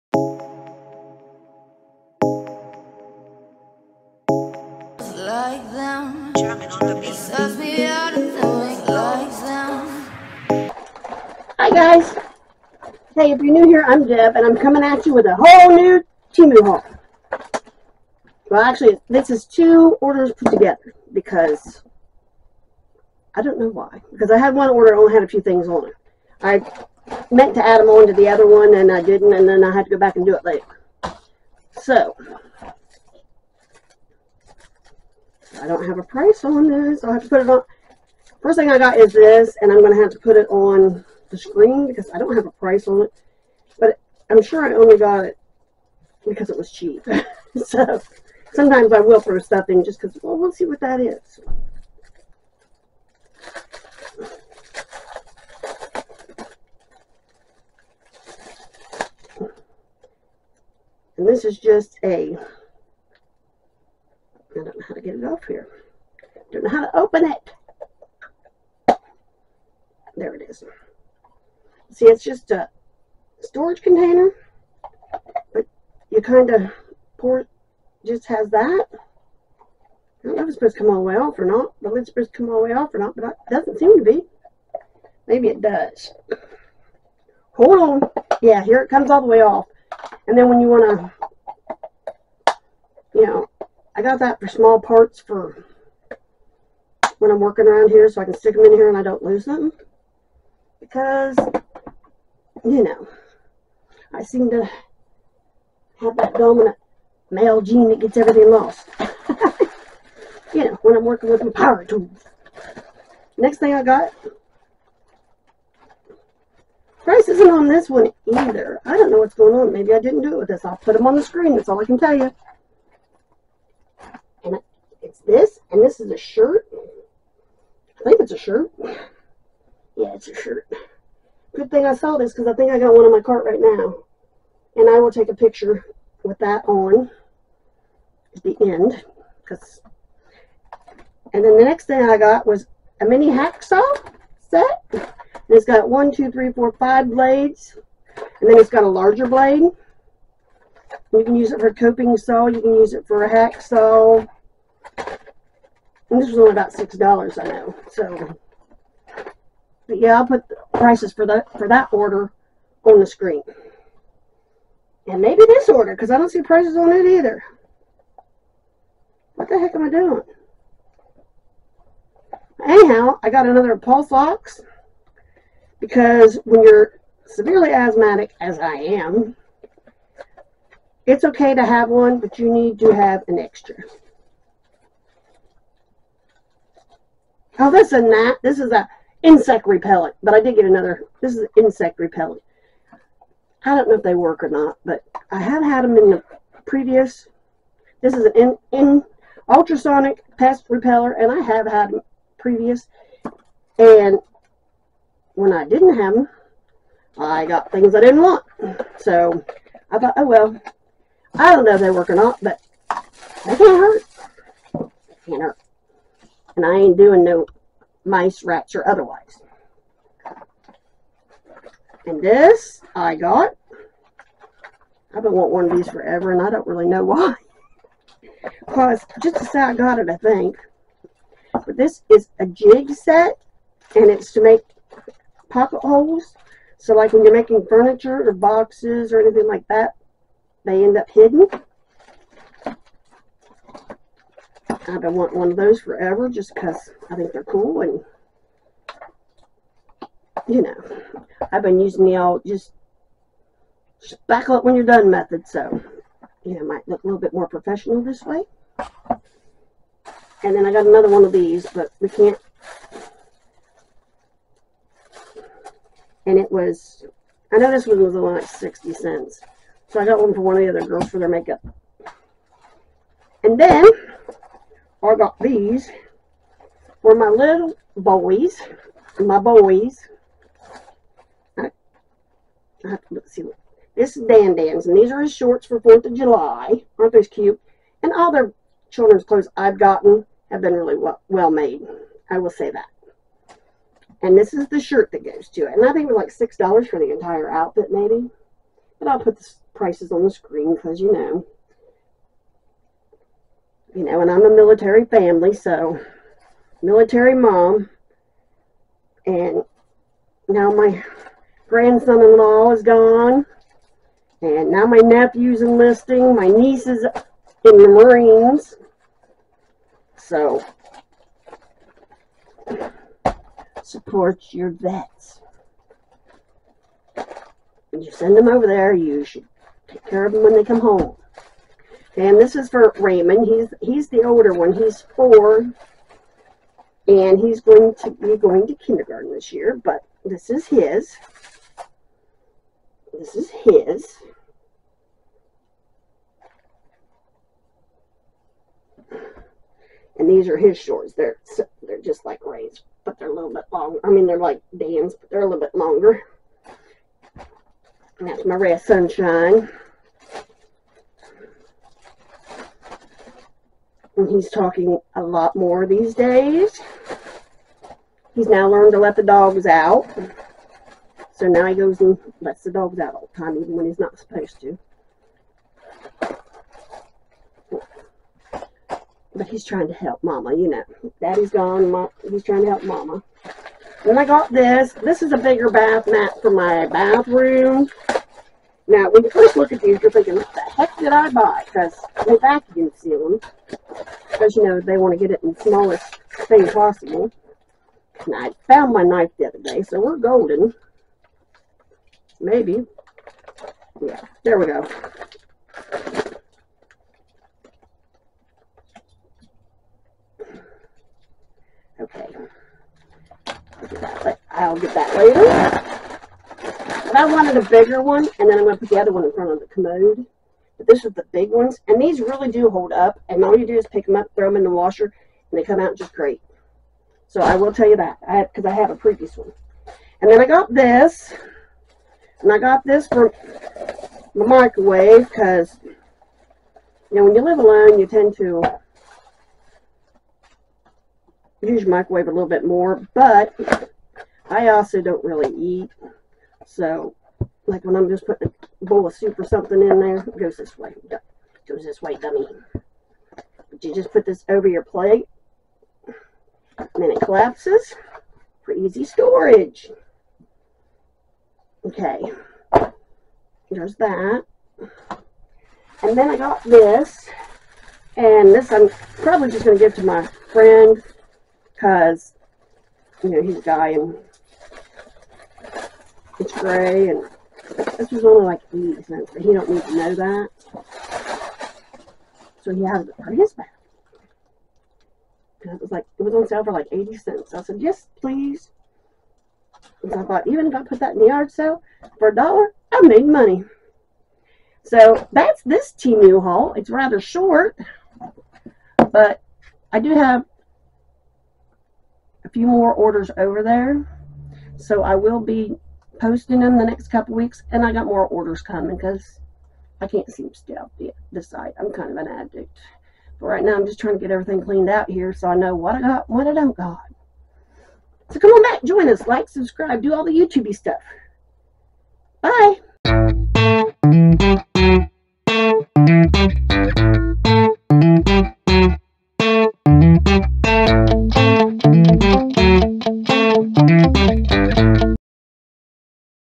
Temu I Love You!
Temu is a shopping site that also has an app. They ship all your order together from California. My first order came within 10-14 days. All my items have been well packed. Overall I was very impressed with both the quality and service.

Order #1  3 items for $11.56

Floral Print Chain Cami Top, Sexy Sleeveless Summer V Neck Vest, Women's Clothing-$7.97

8 In1 Saw Multi Purpose Hand DIY Steel Saw Metal Wood -$5.59

Order #2 had  items for $42.78

Pest repellent-$1.59 sold-out

1pc Memory Foam Bath Rug, Cobblestone Embossed Bathroom Mat-$.78

Simple Letter Print Carry-On Pouch, Zipper Lightweight Coin Purse, Portable Travel Makeup Organizer-$1.87

14pcs Woodworking Tools Pocket Oblique Hole Screw Jig Locator Drill Bits 15 Degree Angle Drill Guide Set Hole Puncher Carpentry Tools-$8.67

Boys Independence Day "Freedom" Outfit Shorts & Vest Sleeveless Crew Neck Casual Summer Kids Clothes-$7.59

Boys "Happy 4th Of July" Stars And Stripes Print Casual Outfit-$7.59

1pc Chenille Shaggy Bath Rug, Door Mat, Efficient Water Absorption-$13.76

I'd love to hear from you! I'd enjoy hearing your story too.
Mailing address:

Debra Neel
Jacksonville,  OH. 45740